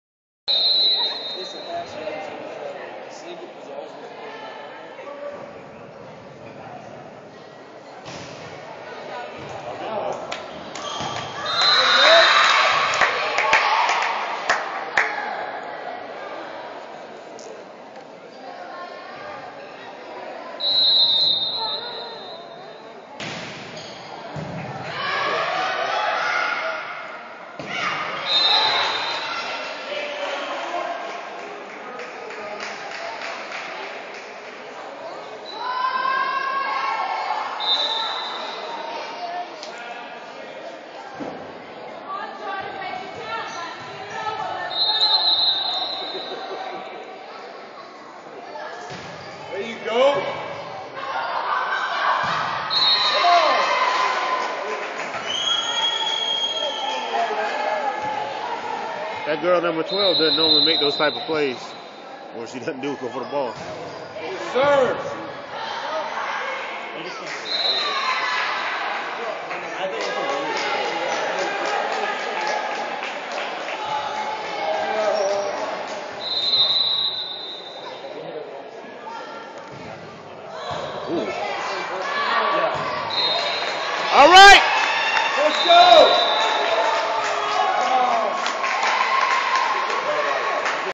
71st vs South View Senior Night pt 3
The Lady Falcons of 71st played spoiler to the Lady Tigers of South View on their final home game of the season.  They Falcons bested the Tigers 3-0.  Preparing for the Mid-South 4A Conference Volleyball Playoffs.